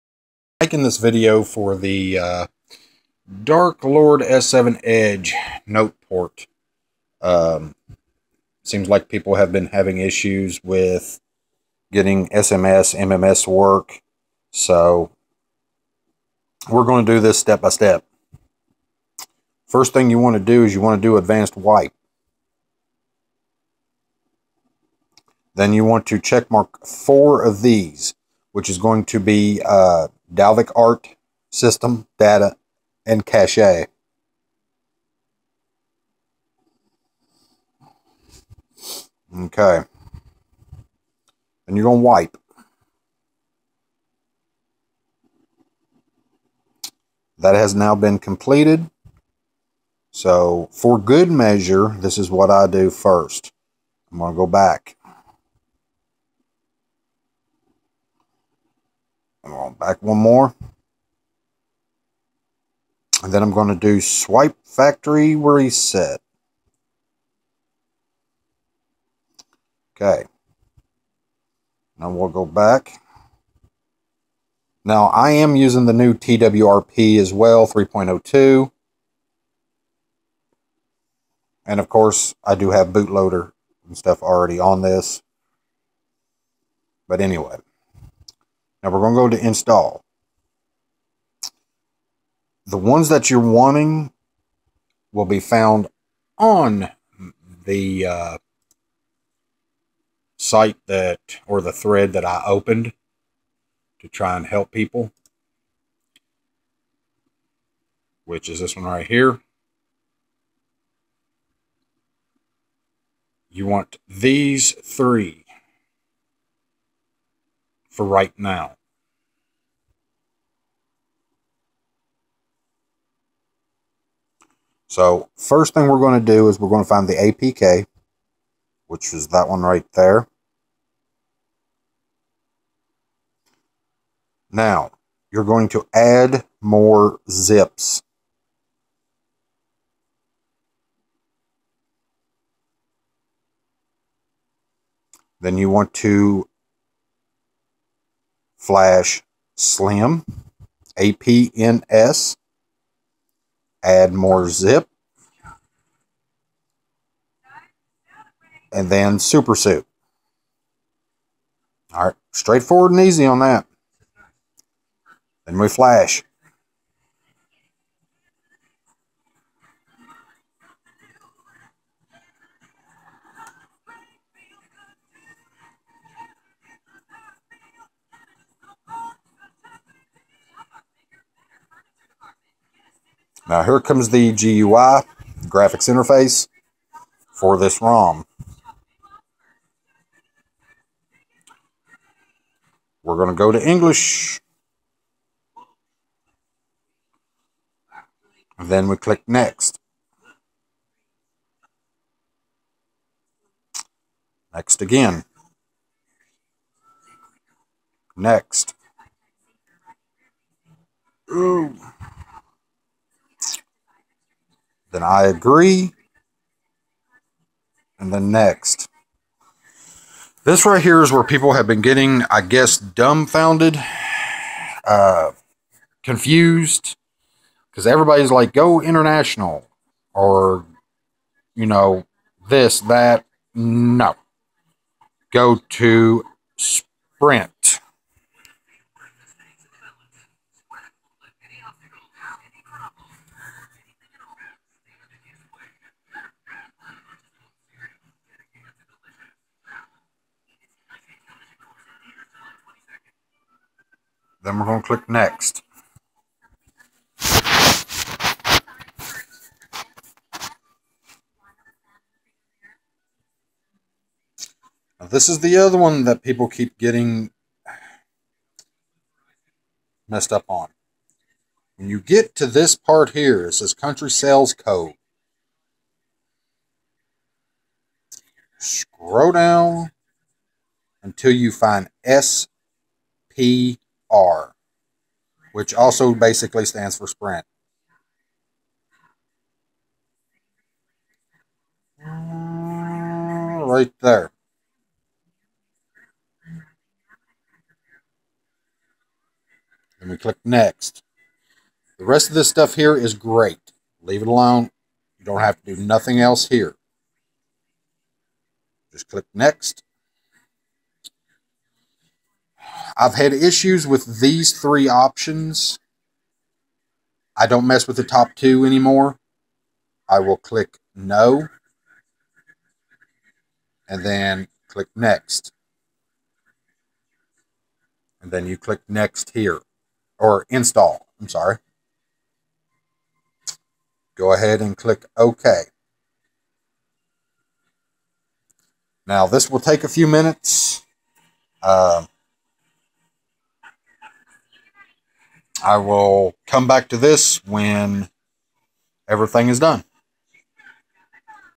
DK s7 install
How to install 3.3 Rom with working SMS/MMS Verizon Note 3